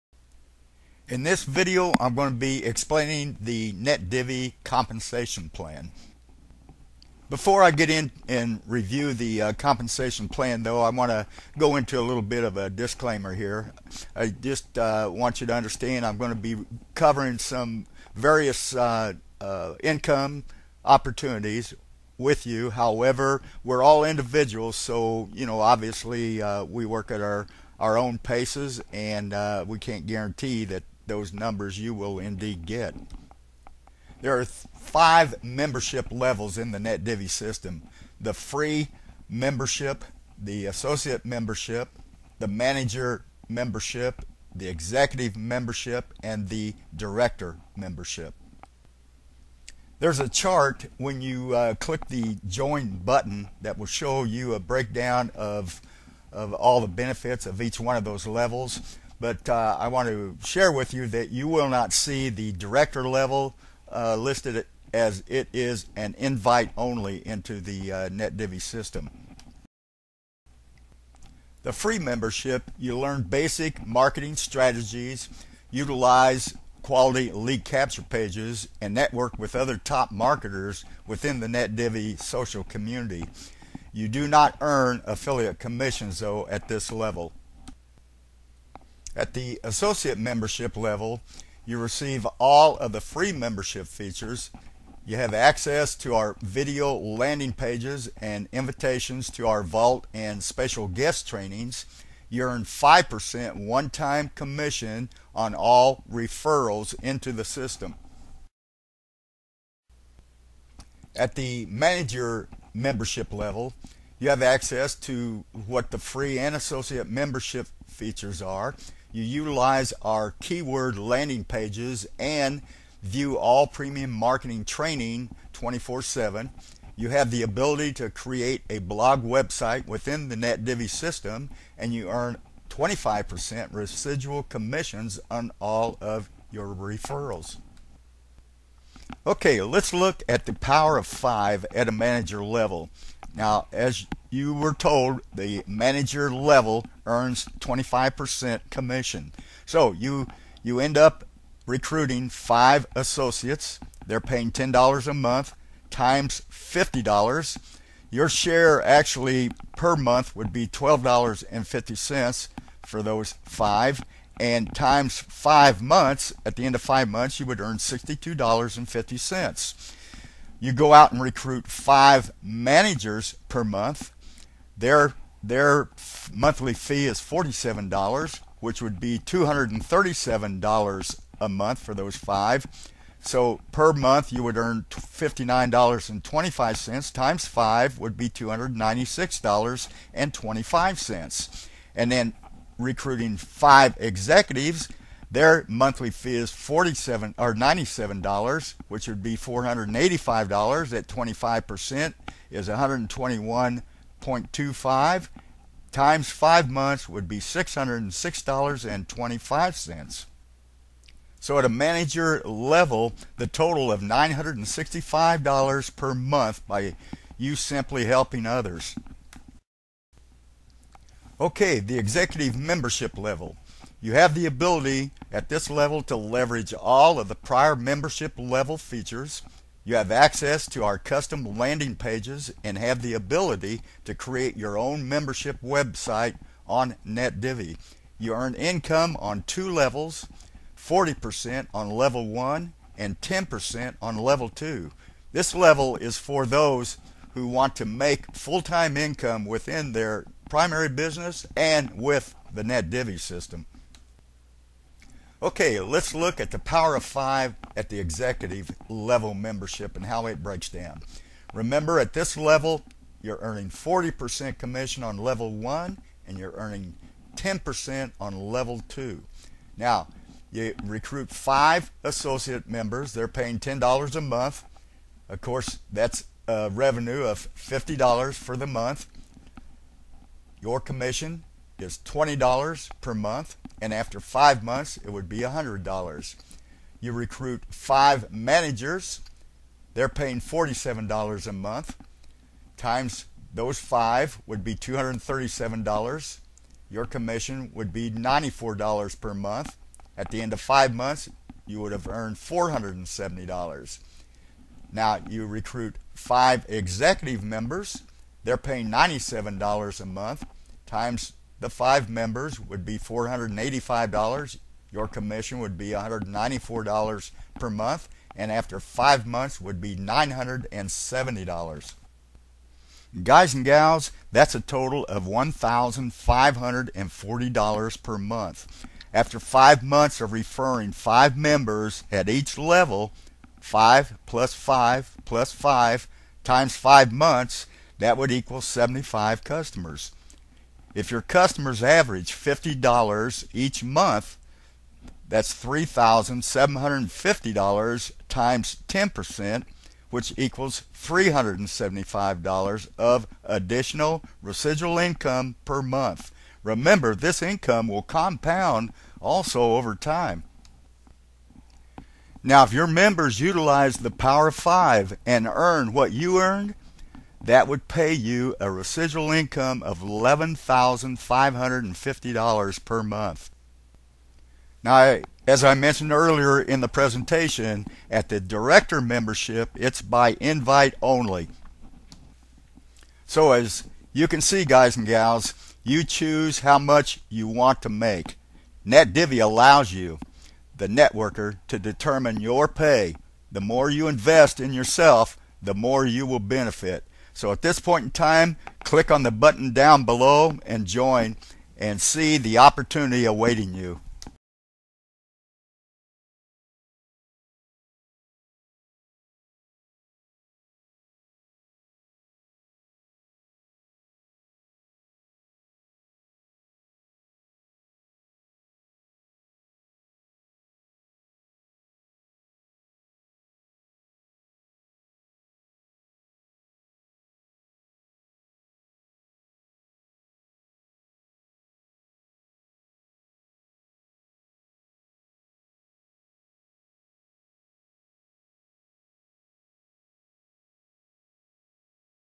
NetDivvy System Compensation Plan
The NetDivvy System has 5 membership levels with up to 40% commission at the 'Executive' membership level. The system works in conjunction with ANY online business. With SEO friendly landing pages, you are taught how to drive targeted traffic into your own sales funnel.